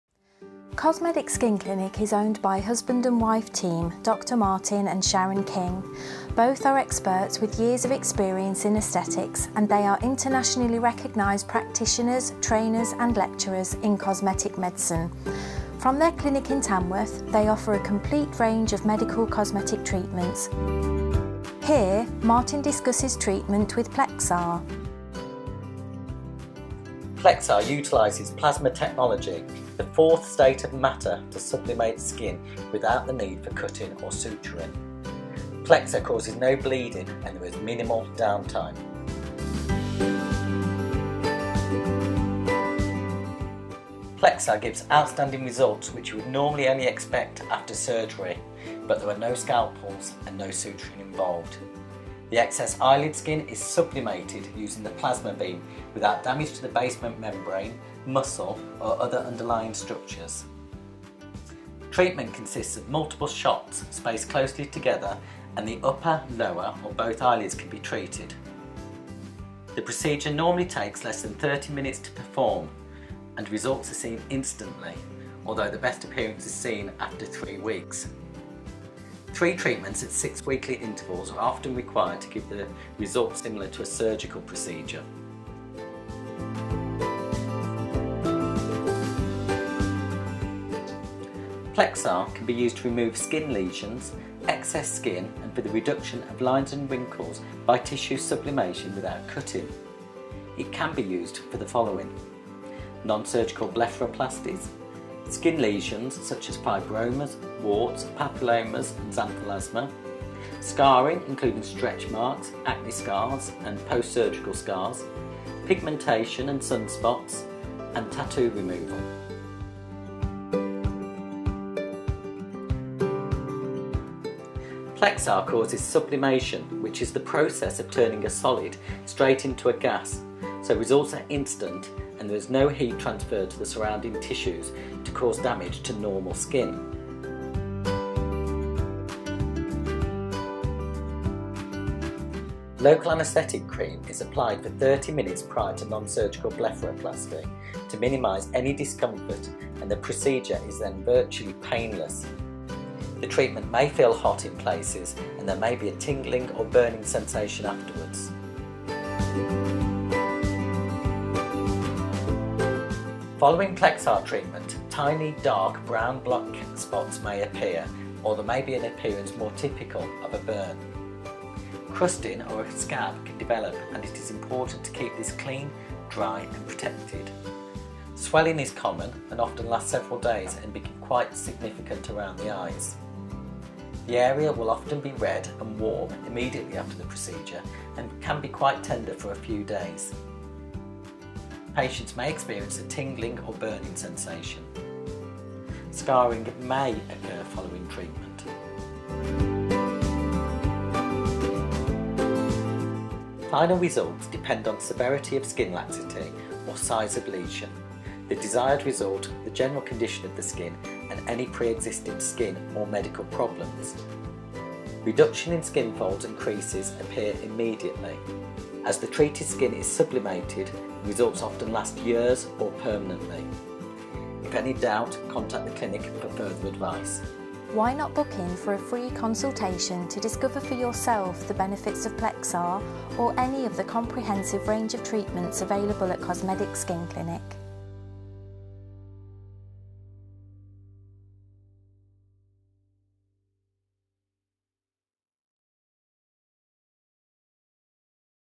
PLEXR Non-Surgical Blepharoplasty
PLEXR for non-surgical blepharoplasty (eye lid surgery) and skin lesion removal. PLEXR utilises plasma technology, the fourth state of matter, to sublimate skin without the need for cutting or suturing. PLEXR causes no bleeding and there is minimal downtime.